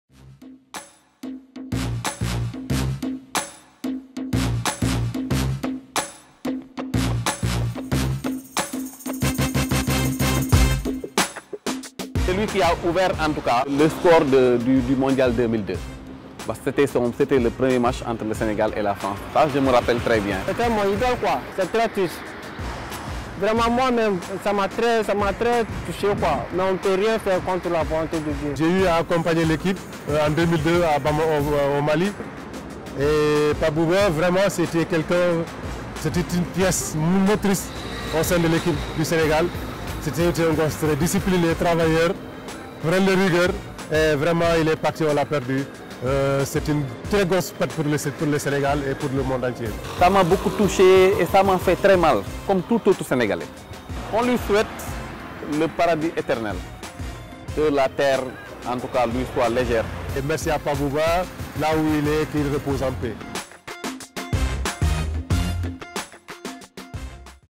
Pape Bouba Diop : des Sénégalais parlent des souvenirs qu'ils gardent du footballeur décédé
Des Sénégalais parlent des souvenirs qu'ils gardent de Pape Bouba Diop.

Ils disent regretter le décès de celui qui a été " la pièce motrice" de l'équipe nationale de football à une époque.

Des propos recueillis par Alassane Dia.